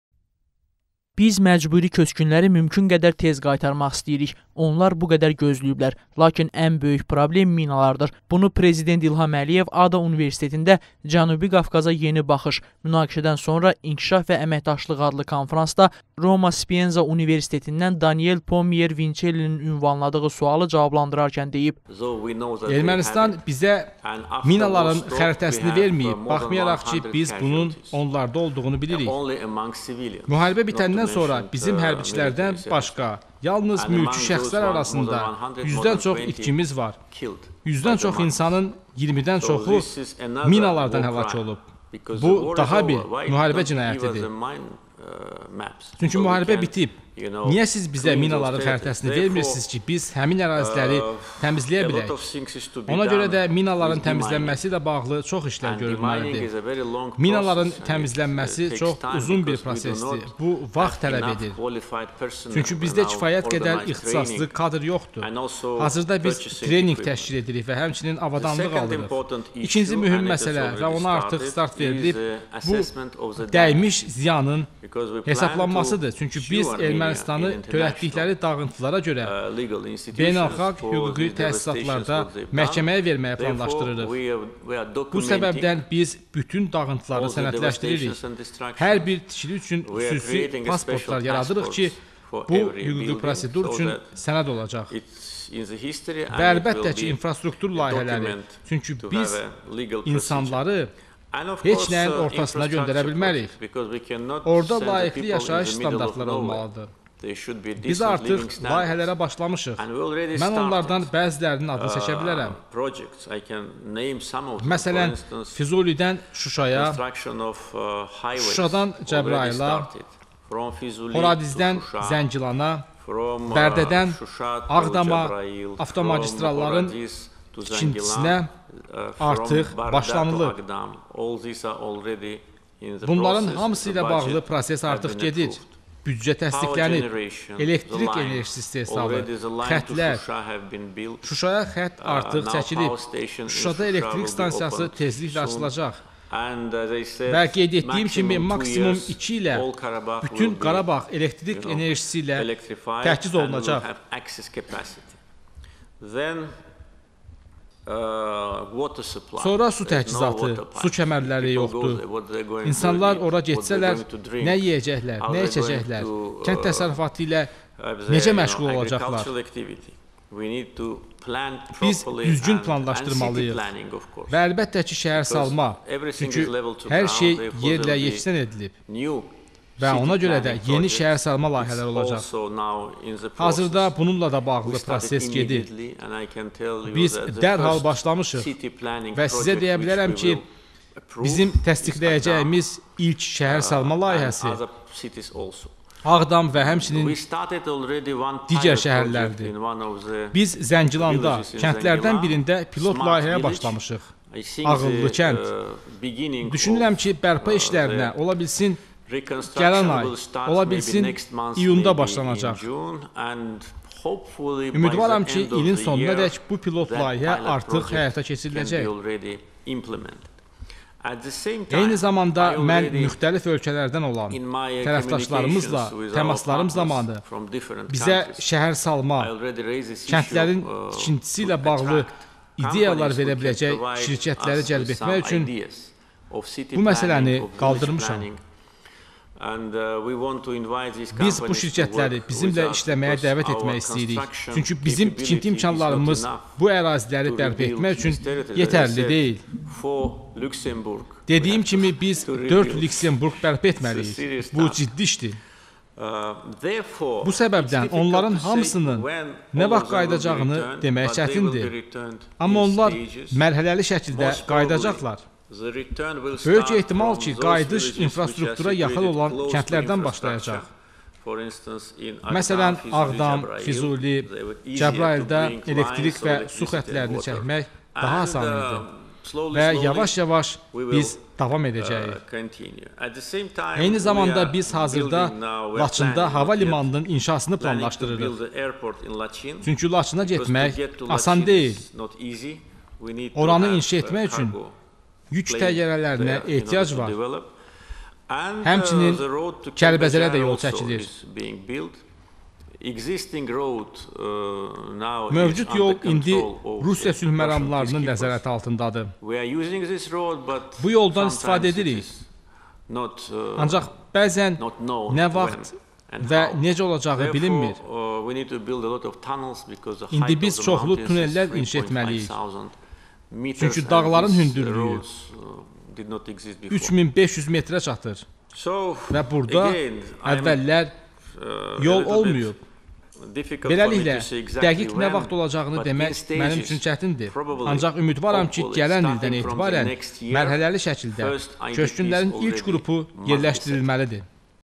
Qarabağa mərhələli qayıdış
İ.Əliyev:"Minaların təmizlənməsi çox uzun bir prosesdir"